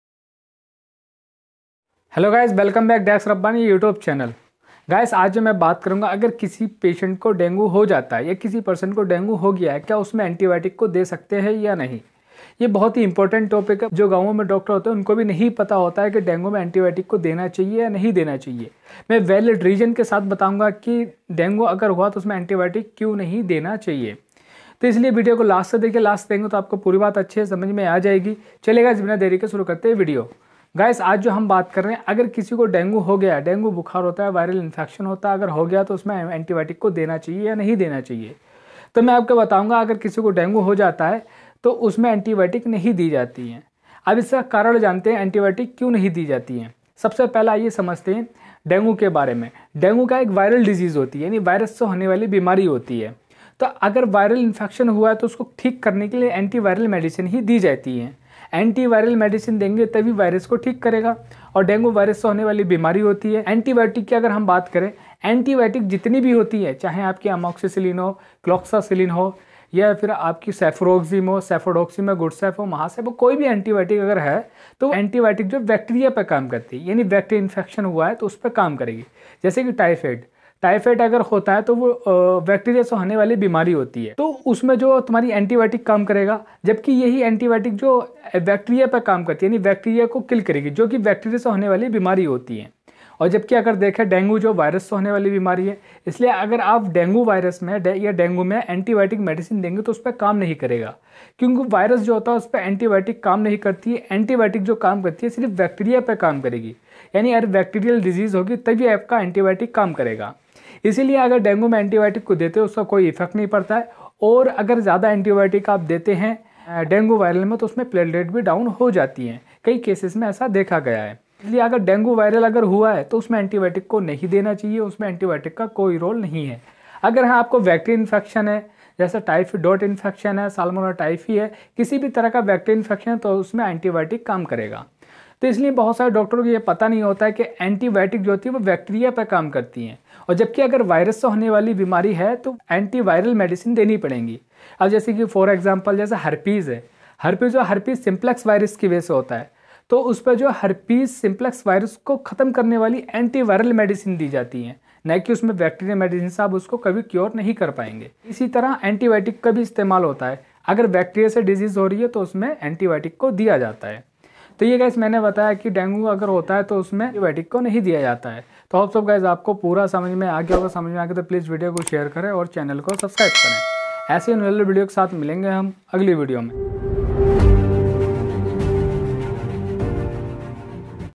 डेंगू में एंटीबायोटिक क्यों नहीं दी जाती है?? || dengue fever treatment | antibiotic in dengue fever
Hi guys

Is video me batne bala hu antiviral disease ke bare me. Or dengue me antibiotic kyu nhi di jati h.
I made videos only education and knowledge pupose. I never be advise self medication.

डेंगू में एंटीबायोटिक क्यों नहीं दी जाती है??

                       Ok_VlDJwAmTcp5-KIhjg&s=09